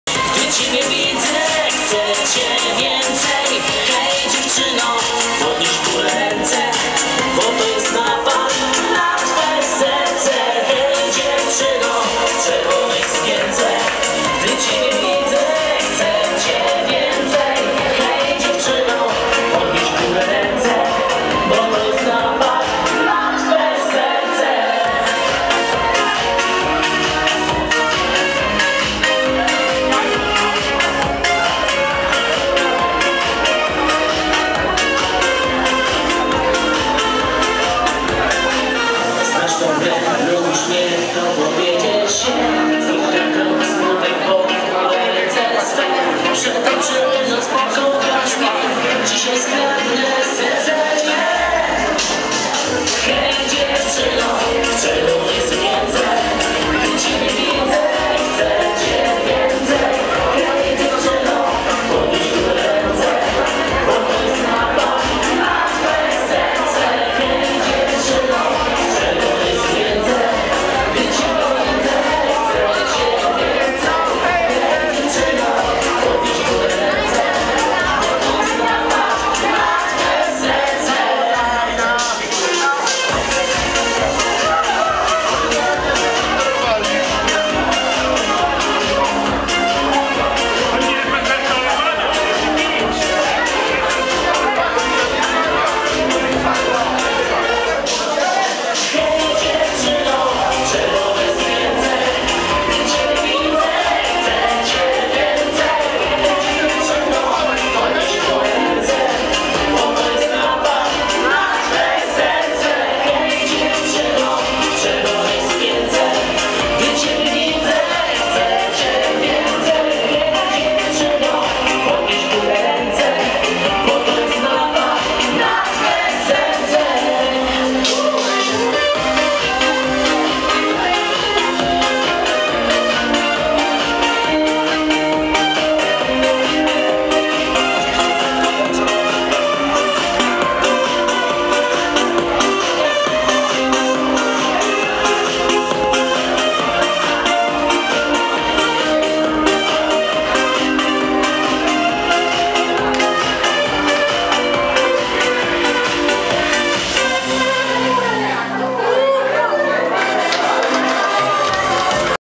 Sylwester 2017/2018 w Edenie
Ach, co to był za Sylwester! Muzyka i tańce do białego rana, fajerwerki i wspaniała atmosfera. Tak zapamiętamy ostatni bal w Edenie w Suchocinie. Jeszcze raz dziękujemy naszym Gościom, że mogliśmy wspólnie pożegnać 2017 rok i przywitać 2018. Kolejna okazja do zabawy już 10 lutego. Właśnie wtedy organizujemy Bal Ostatkowy.